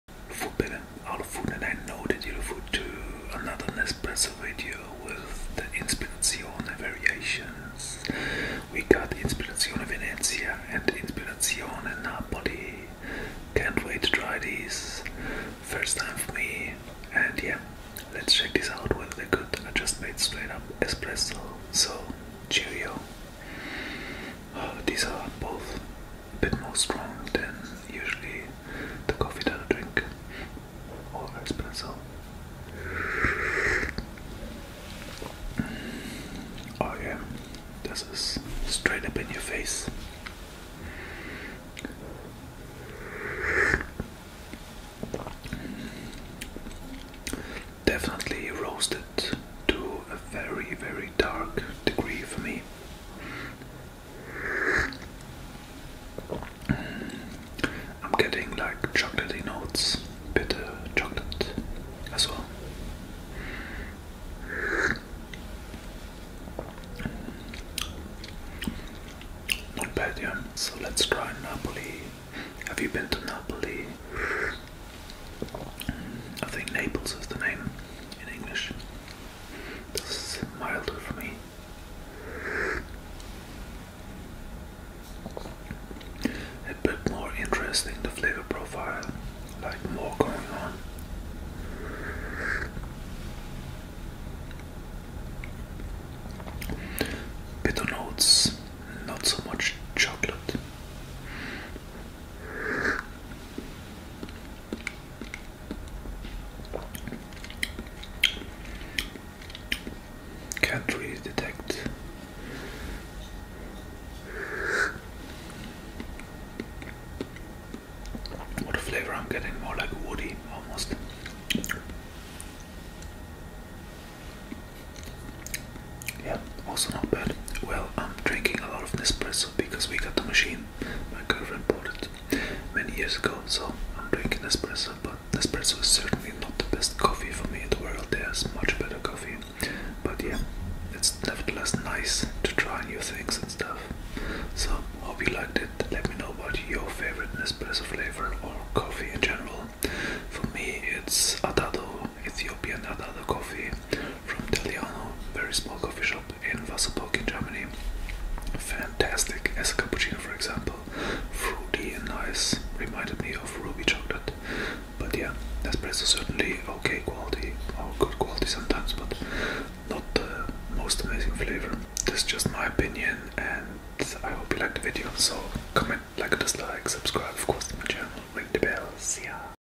ASMR Drinking Sounds - Nespresso Inspirazione Taste Test – Venezia e Napoli
In this video you will see two new Nespresso variations from the Inspirazione line: Venezia and Napoli. I tried both as espressos.

Good and interesting coffee review.

What is your favorite drink?